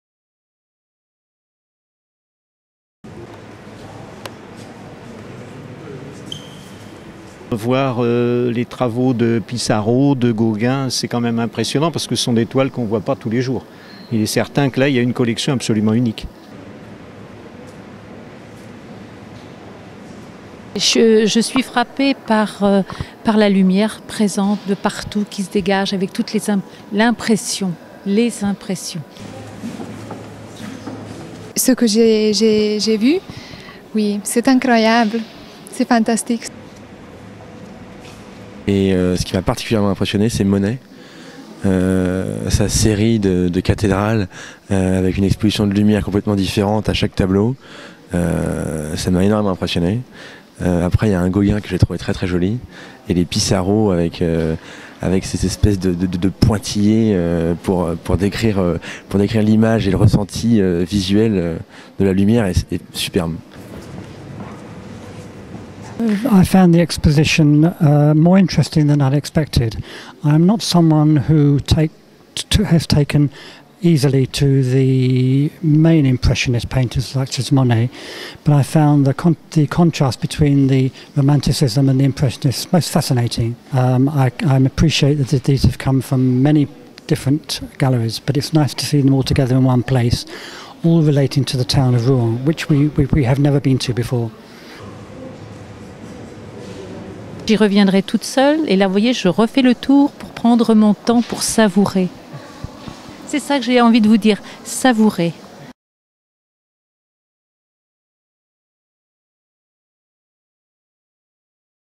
Une ville pour l'impressionnisme
Dans le cadre de Normandie impressionniste, le musée des Beaux-arts de Rouen propose une exposition intitulée "Une ville pour l'impressionnisme".

Déçus ou pas déçus ? Les visiteurs donnent leurs "impressions".